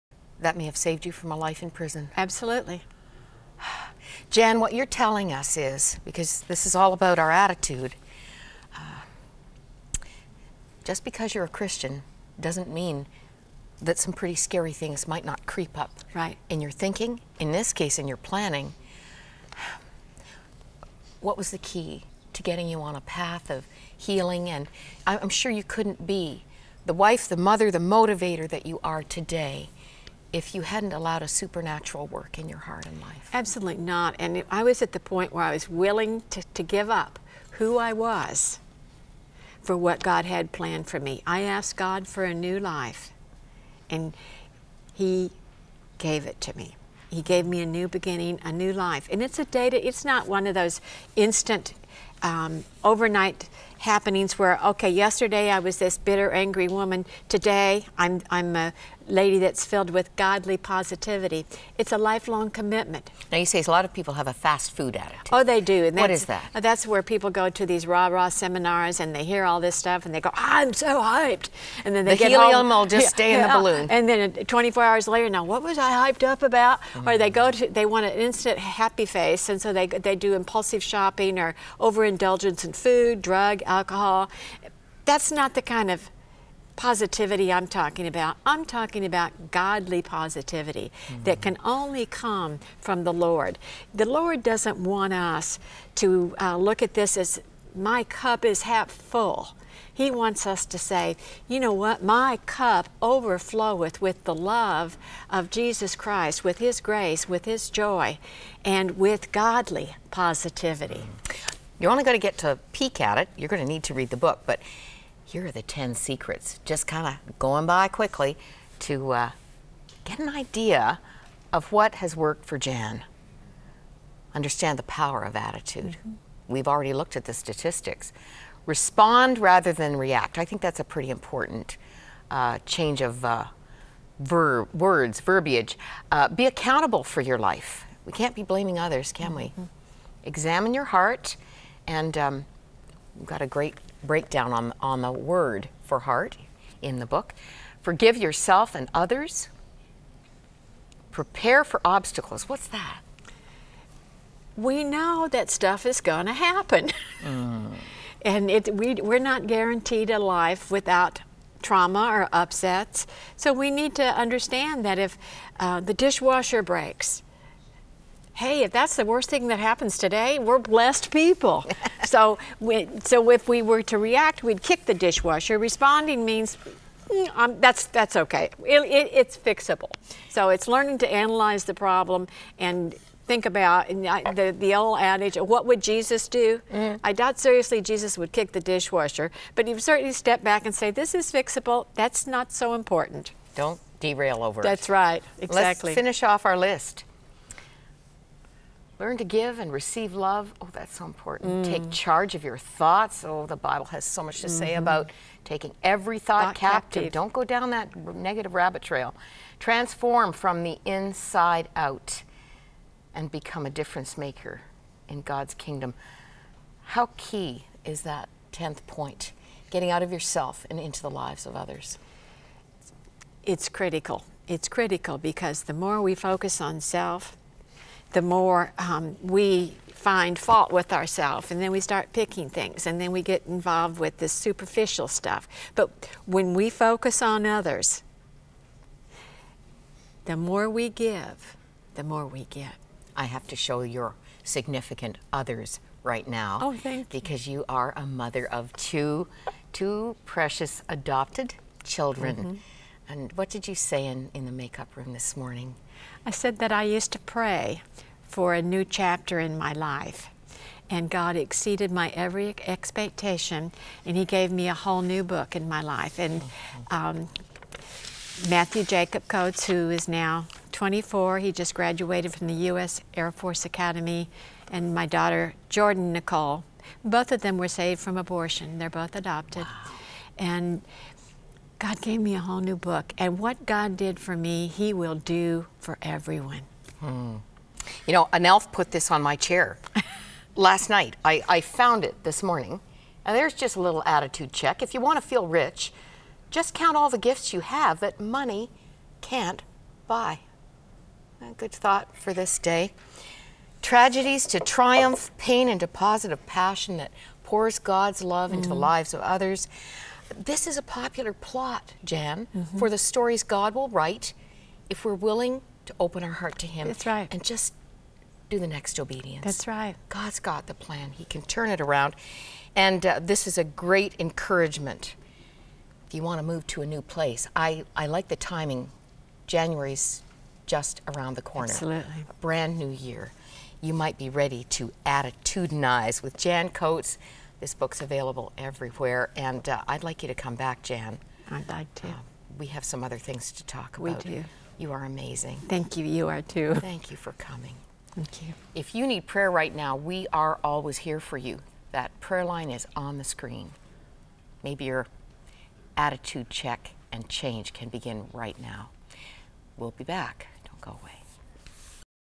"Son Was Killed: Forgiveness & New Life" -- Jan Coates "Attitude-inize" 2/2
Jan Coates is an author and motivational speaker. She has overcome childhood abuse, divorce and became a very successful business woman. When Jan's son was murdered by a drunk driver her desire for revenge almost led to murder. Today Jan is a a living testimony that God loves, forgives, redeems, and then transforms us into a new creation.